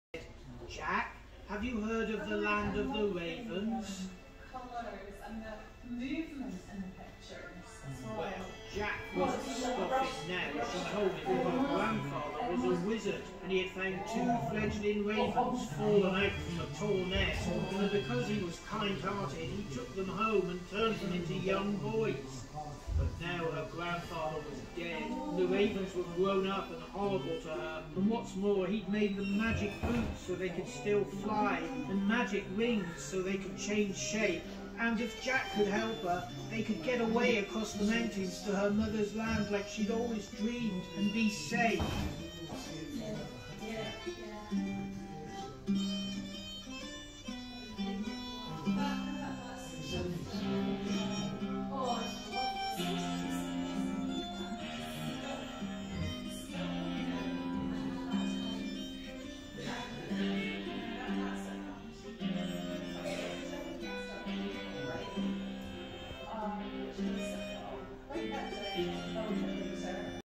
John Slavin Land of the Ravens Exhibition Video
This short film shows the exhibition space in The Story Telling Centre, The Netherbow Theatre in Edinburgh. This is where the John Slavin paintings for the Two Ravens film were shown in October 2022 along with the animated film of the Two Ravens story by John McGeoch. This exhibition film was made by artist Susan Dawson.